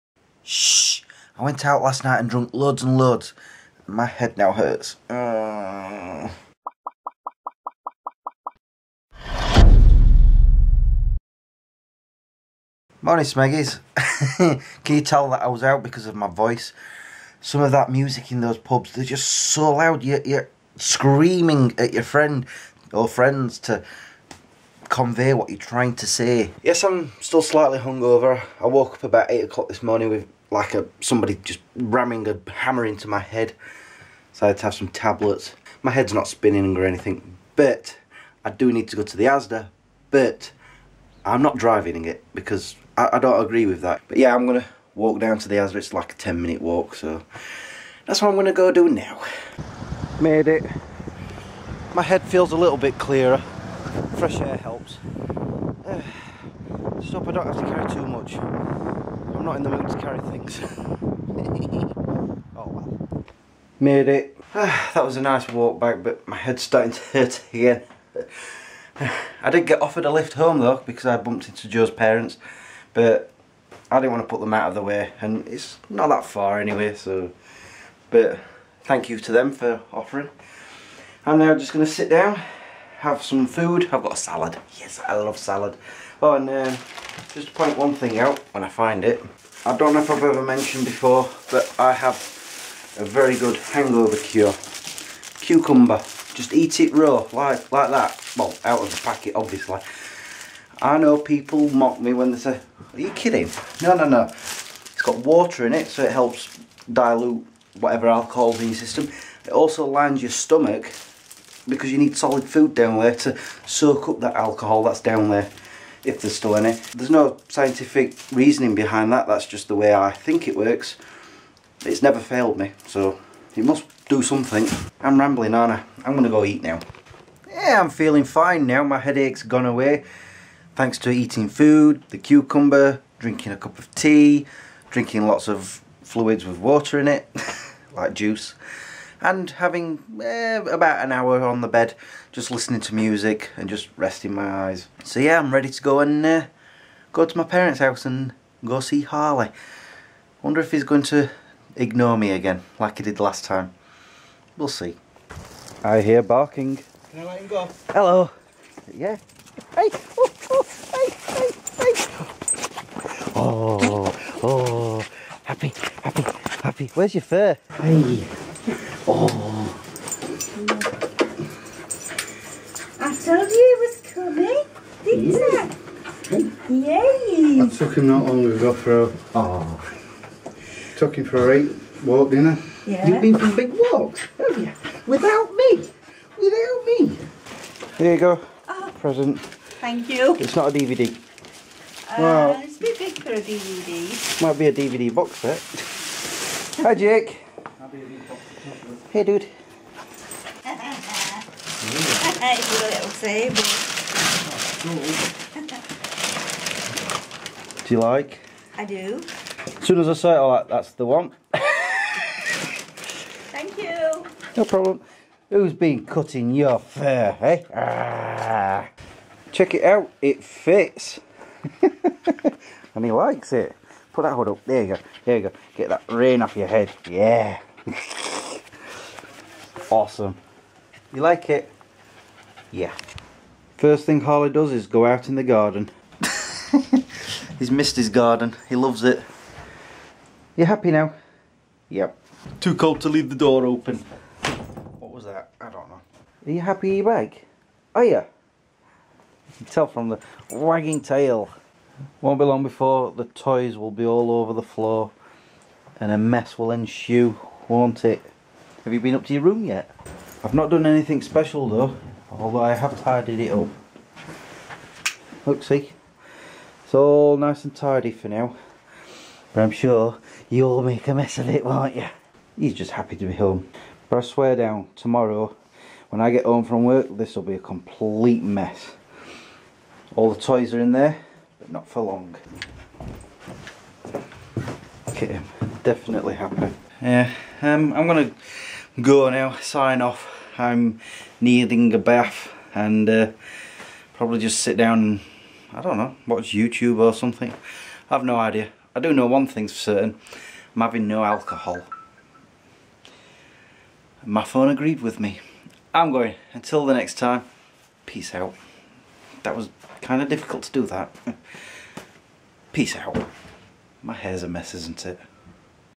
HUNG OVER [Vlog 688]
Went out drinking the night before. Oh, and I pick up Harley!